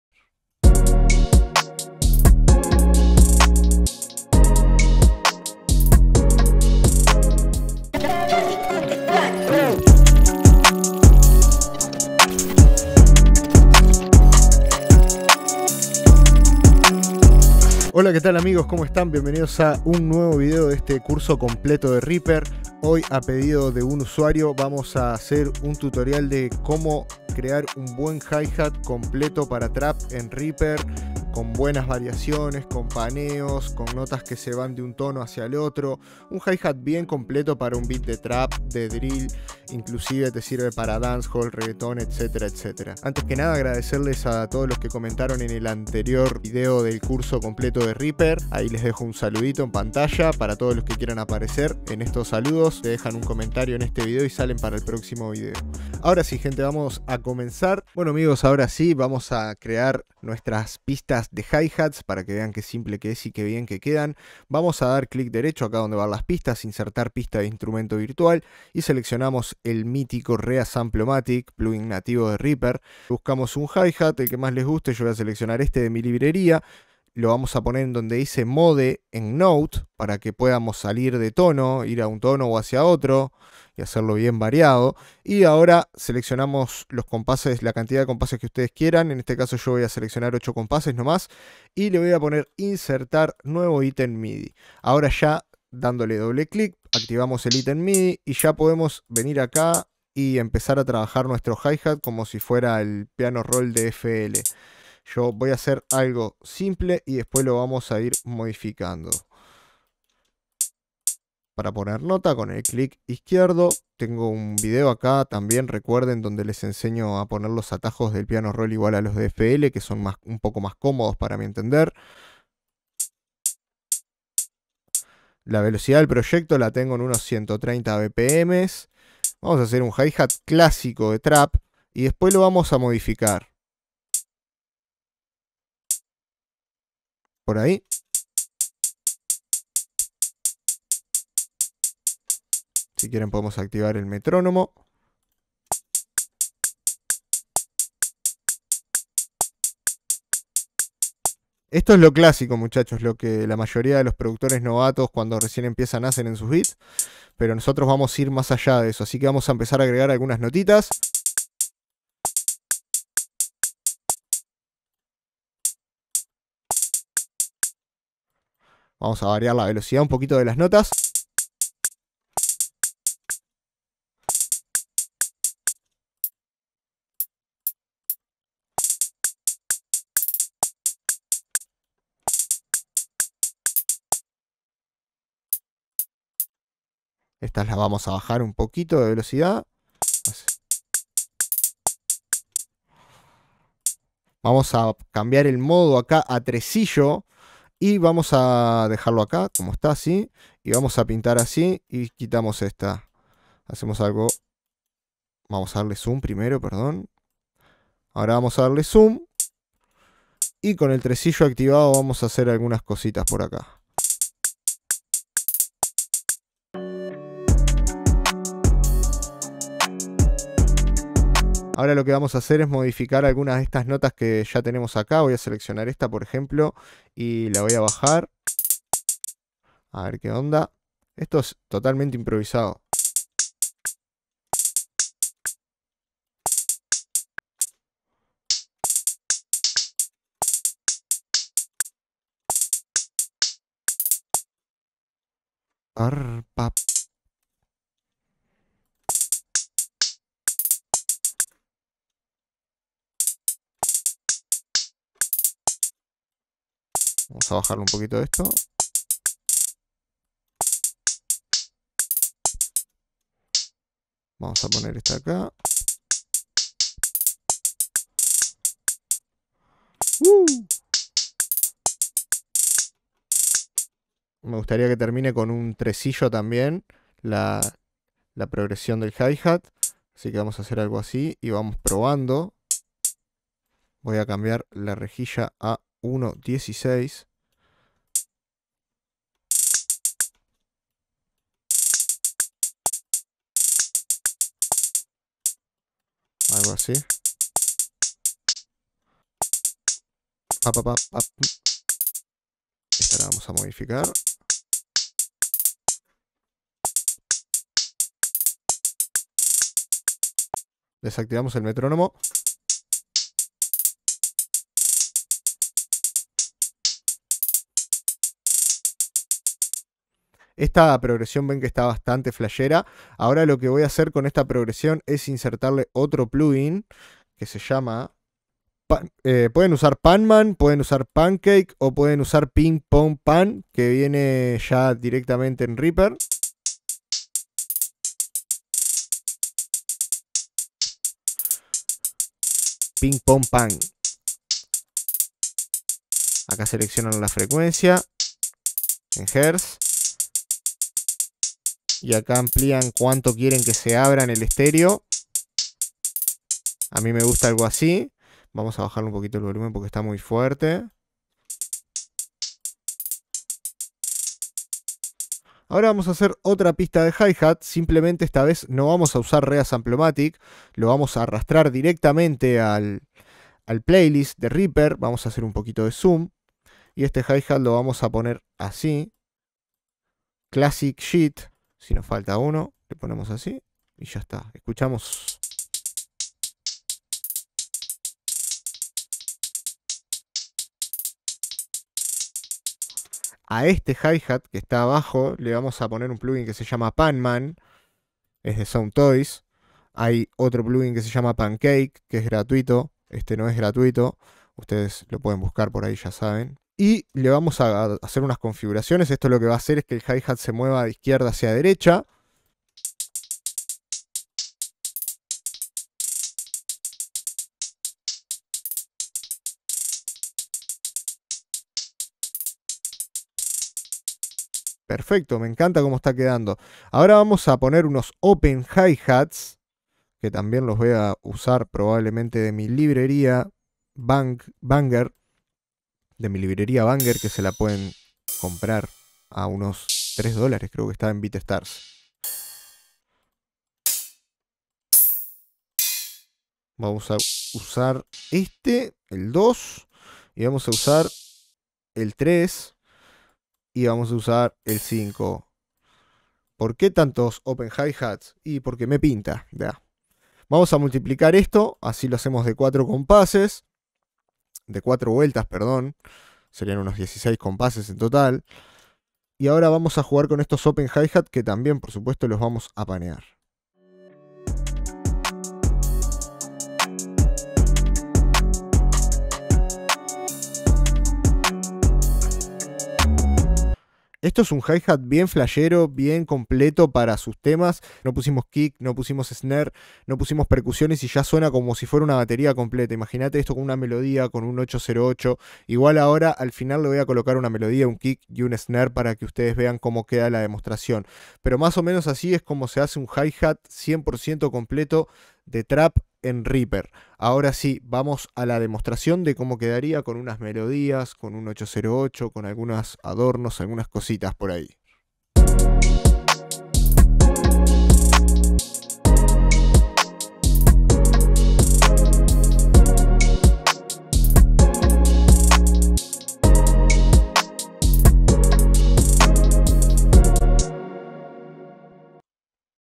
Hi hats de TRAP nivel PRO en REAPER - Curso completo reaper cap 15 - Yardin Beats
En este video te enseño a hacer unos hi hats de 4 layers que suena super bien en TRAP y DRILL ! Como siempre usaremos mi DAW de confianza REAPER !

FACEBOOK FAN PAGE : YardinBeats